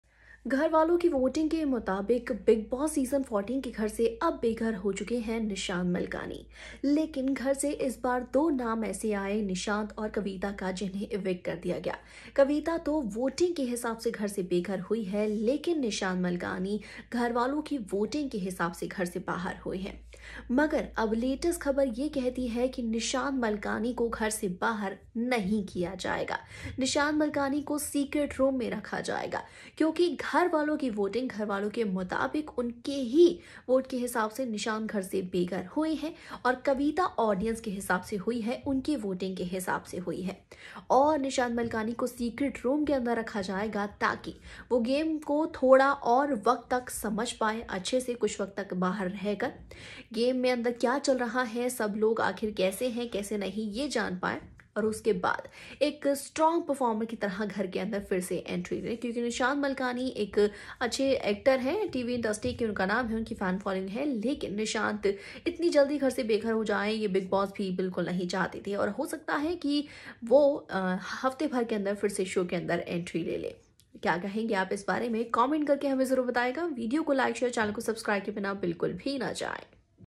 Bigg Boss 14: Nishant Malkani In Secret Room After Elimination, Fake Eviction?
Bigg Boss 14: Nishant Malkani In Secret Room, Fake Eviction?